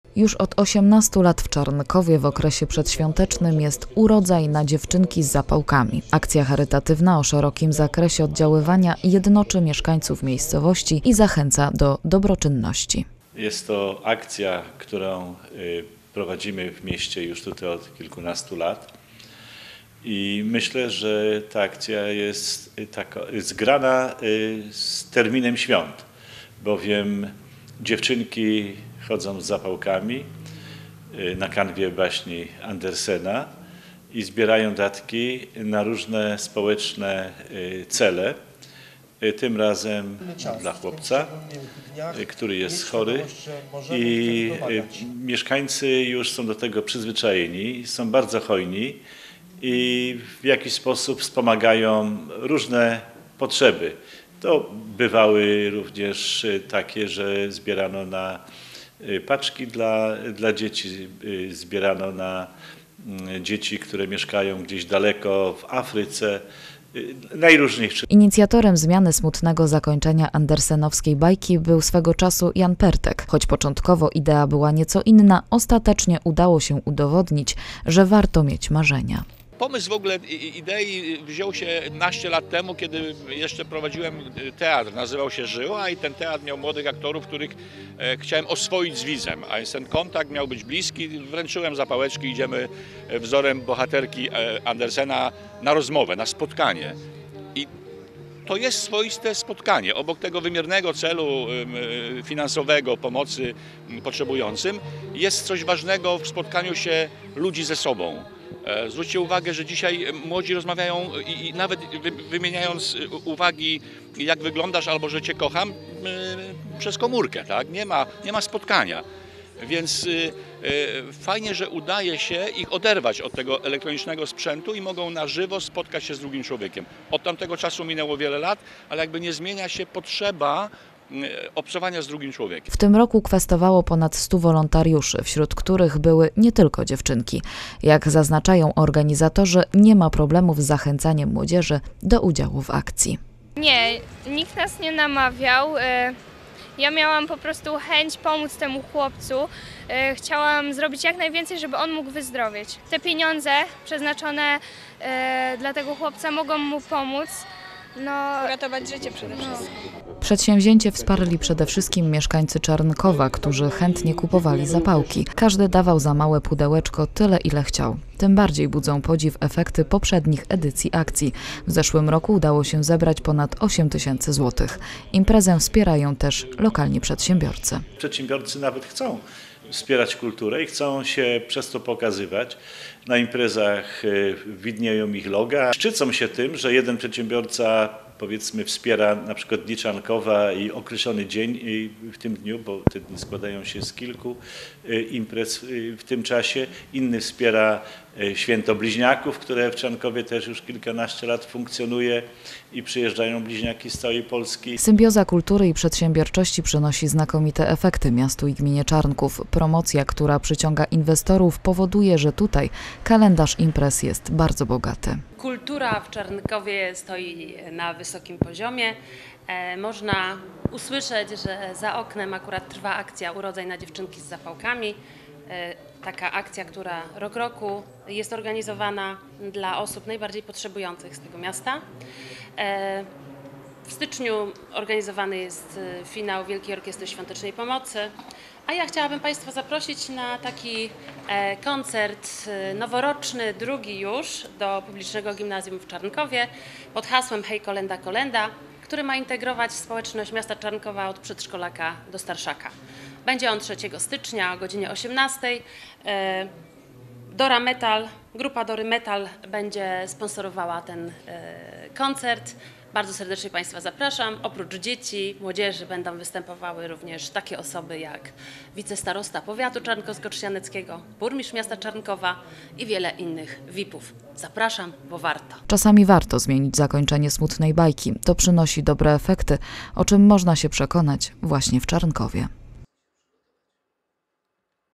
"Dziewczynki z zapałkami" - coroczna akcja charytatywna w Czarnkowie - 22.12.2015.
Realizacja reportażu: TV WIELKOPOLSKA